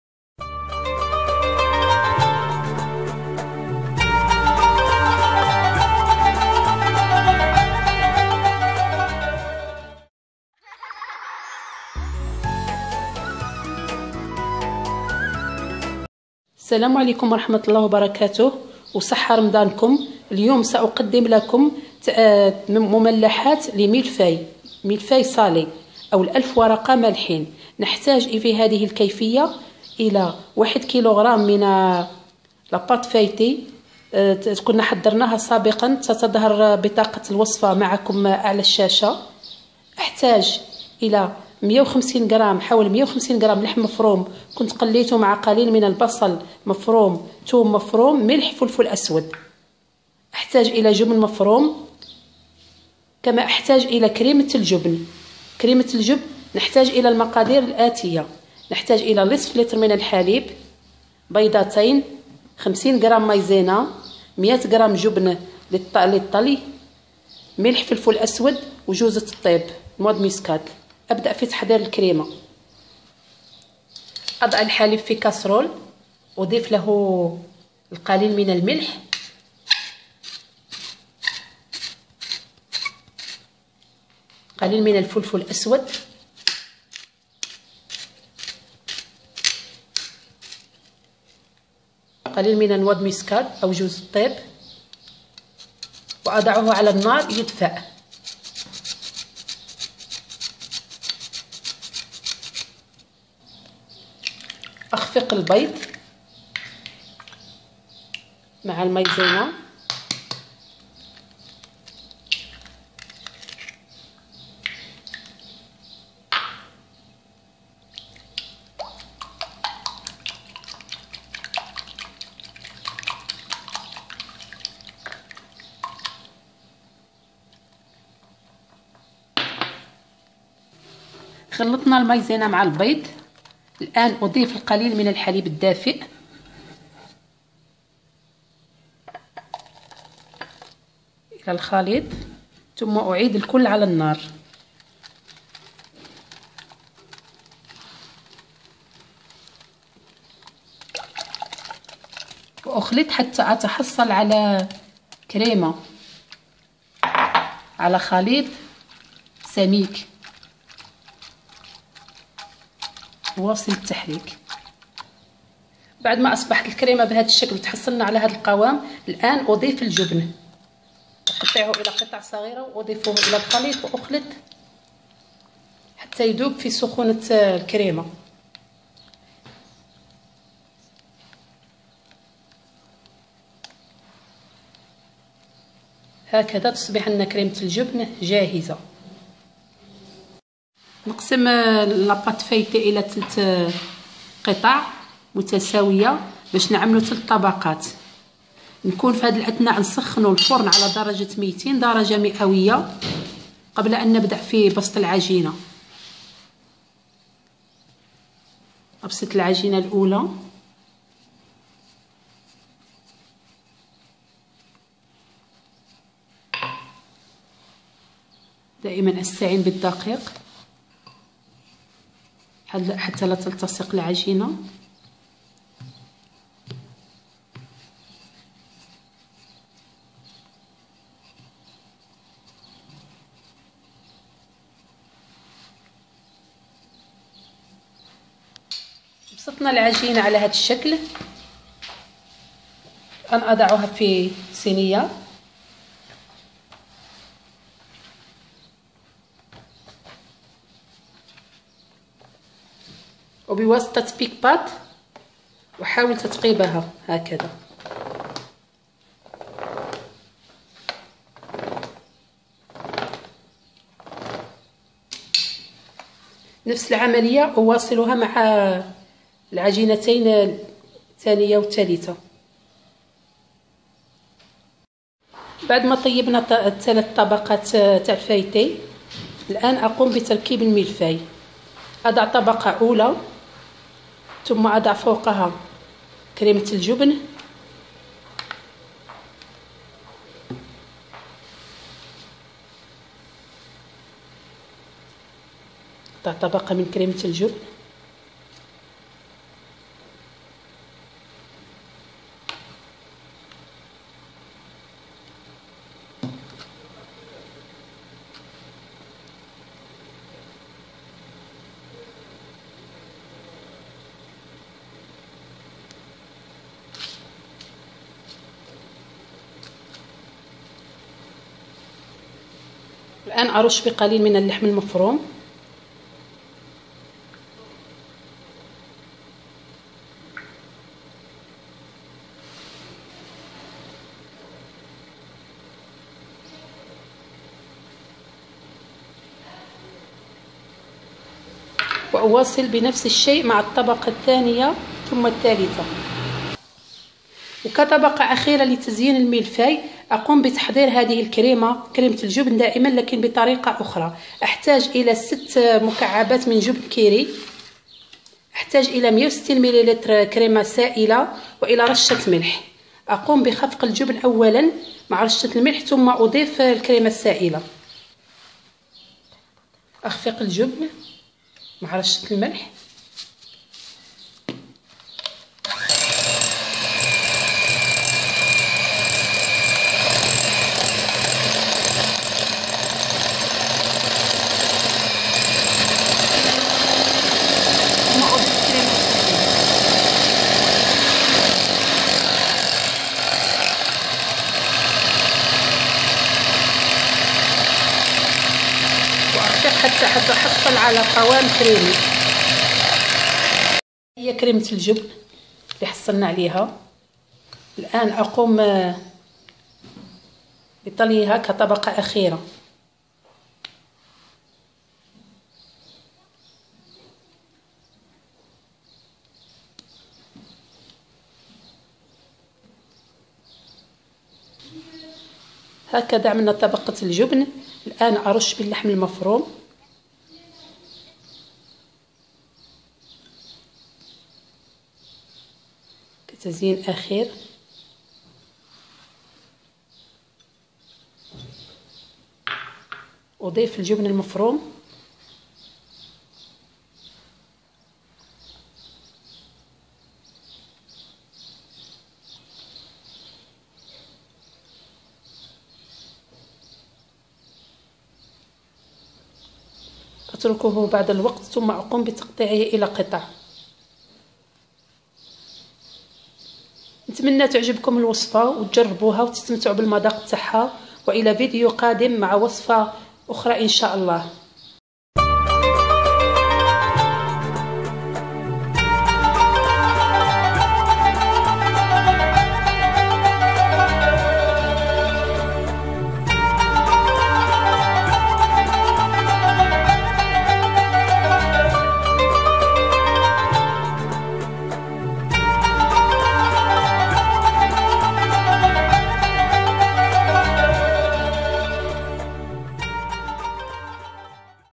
ألف ورقة مالحة -mille feuille salé
زورني على الفيس بوك / selsabil cuisine
Ingrédients :
1 kg de pâte feuilletée
Farce :
150 g de viande hachée cuit dans
3 cuillères à soupe d'huile
1 oignon coupé finement
2 gousse d'ail coupé finement
Sel
Poivre noir
Crème au fromage :
1/2 litre de lait
2 oeufs
50 g de maïzena
100 g de fromage Fromage à tartine
Noix de muscade
Sel
Poivre noir
Décoration :
Crème au fromage =
6 portion de fromage kiri
160 ml de Créme liquide
Une penci de sel
Fromage râpé